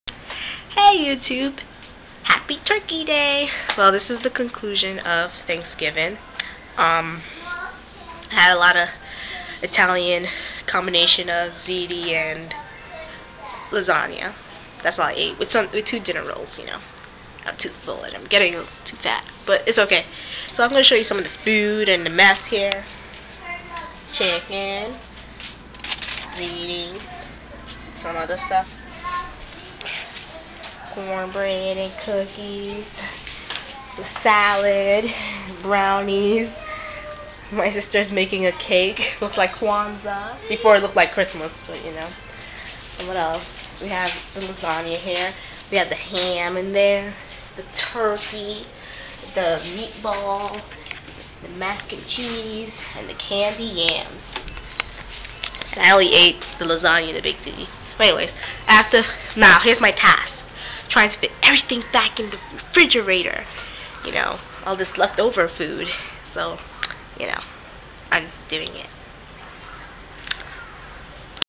Thanksgiving FOOOD!
here's a dumb video with me showing you guys my thanksgiving food!